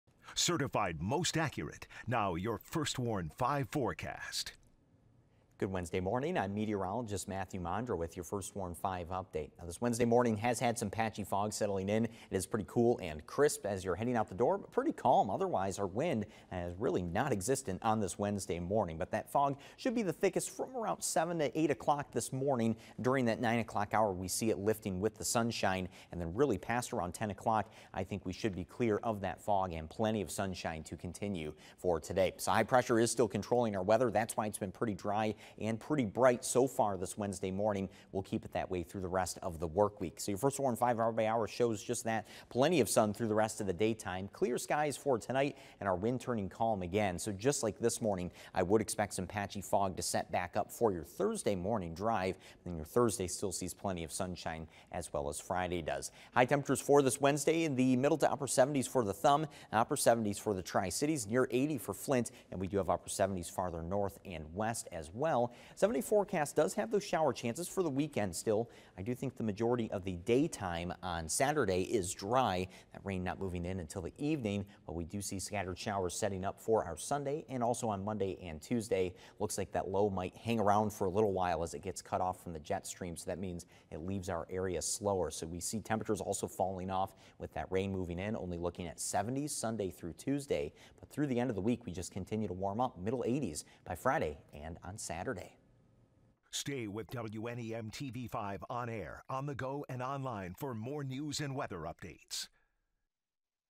First Warn 5: Wednesday Morning Forecast, Sept. 7th
Meteorologist Mathieu Mondro has your Wednesday morning forecast.